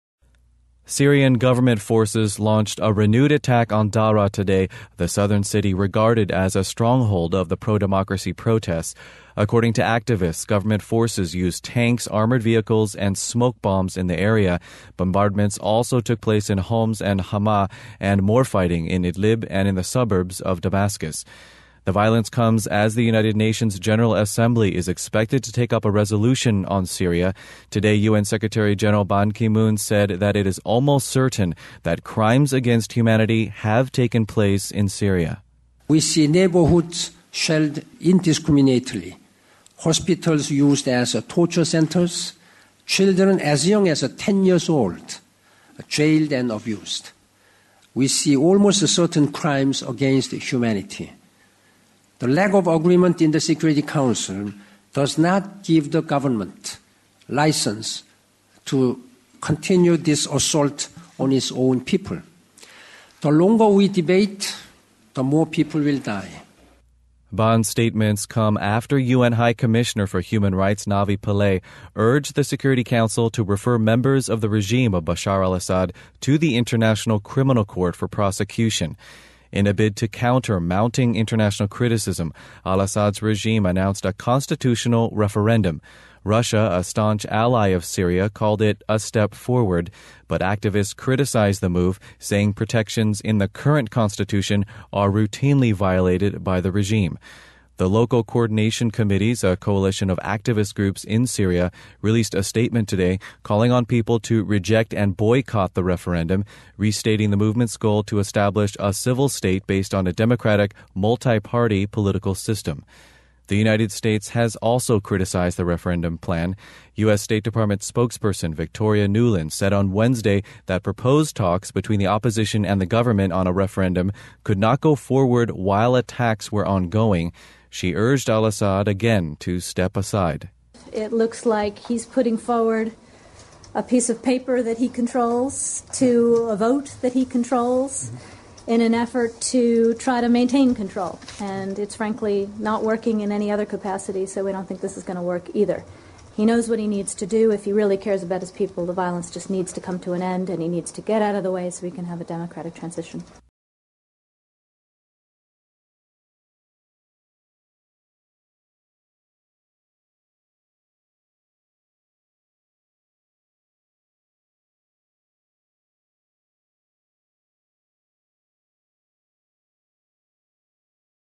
FSRN Troops Attack Daraa Whilst Constitutional Referendum is Being Prepared
Syrian government forces launched a renewed attack on Daraa today, the southern city regarded as a stronghold of the pro-democracy protests. According to activists, government forces used tanks, armored vehicles and smoke bombs in the area. Bombardments also took place in Homs and Hama, and more fighting in Idlib and in the suburbs of Damascus. The violence comes as the United Nation's General Assembly is expected to take up a resolution on Syria. Today, UN Secretary General Ban Ki Moon said that it is almost certain that crimes against humanity have taken place in Syria. "We see neighborhoods shelled indiscriminately. Hospitals used as torture centres. Children as young as ten years old jailed and abused. We see almost certain crimes against humanity. The lack of agreement in the Security Council does not give the government license to continue this assault on its own people. The longer we debate, the more people will die." Ban's statements come after UN High Commissioner for Human Rights, Navi Pillay, urged the Security Council to refer members of the regime of Bashar Al Assad to the International Criminal Court for prosecution. In a bid to counter mounting international criticism, Al Assad's  regime  announced a constitutional referendum. Russia, a staunch ally of Syria called it "a step forward," but activists criticized the move saying protections in the current constitution are routinely violated by the regime. The Local Coordination Committees, a coalition of activist groups in Syria released a statement today, calling on people to reject and boycott the referendum, restating the movement's goal to establish a civil state based on a "democratic, multi-party political system." The United States has also criticized the referendum plan. US State Department Spokesperson Victoria Nuland said on Wednesday that proposed talks between the opposition and the government on a referendum could not go forward while attacks were ongoing. She urged Al Assad again to step aside. "It looks like he's putting forward a piece of paper that he controls to a vote that he controls in an effort to try to maintain control. And it's frankly not working in any other capacity so we don't think this is going to work here either. He knows what he needs to do if we really cares about his people. The violence just needs to come to an end and he needs to get out of the way so his country he can have a democratic transition."